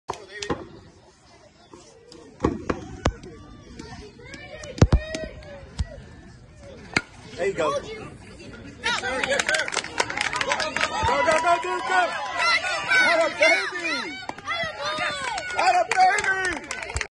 David Handy III- Two RBI One-hopper up the middle @ Backyard Brawl Premier Baseball, Tomball, Tx
Video from Alejandro Martínez RD42